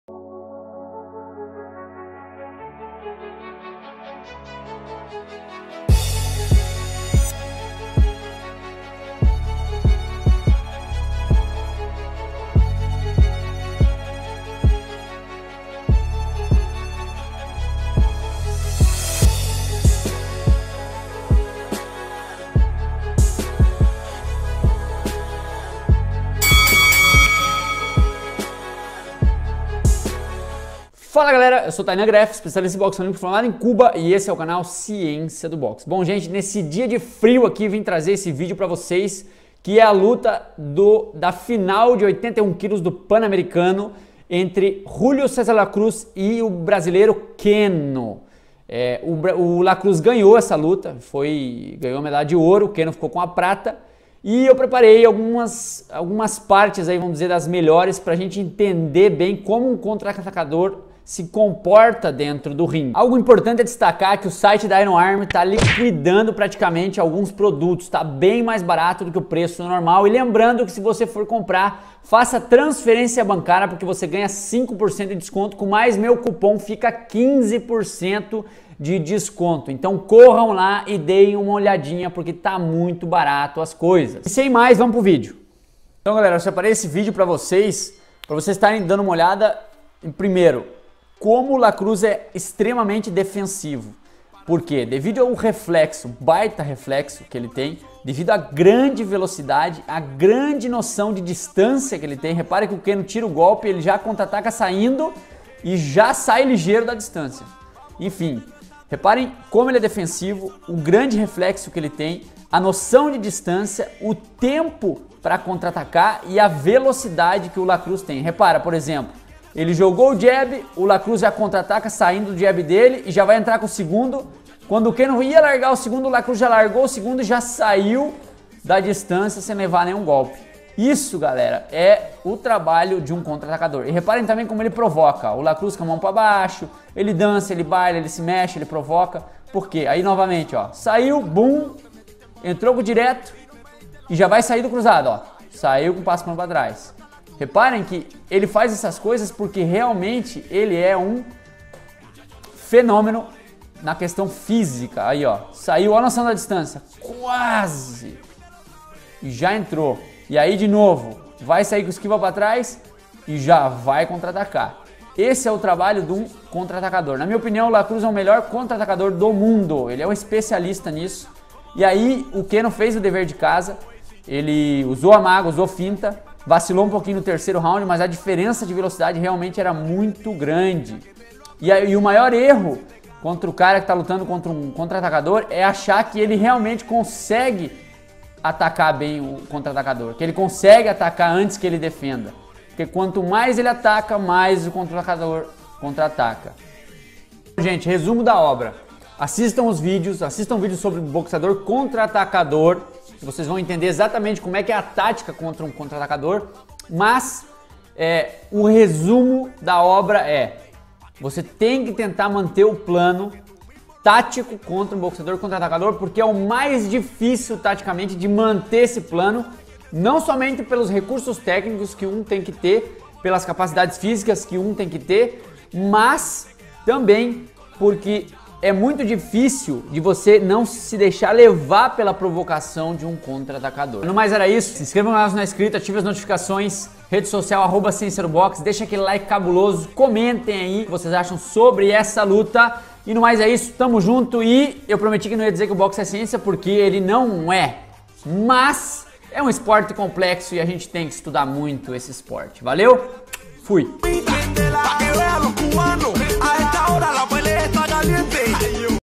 ANÁLISE RÁPIDA - KENO VS LA CRUZ
⠀1 COMENTE NESTE VÍDEO ( Dúvidas e sugestões)

    2 NÃO ESQUEÇA DO LIKE NO VÍDEO ⠀

Galera nesse vídeo FAÇO UMA ANÁLISE RÁPIDA  da luta da final do pan americano de 2019.

Tmj e o Boxe é Ciência.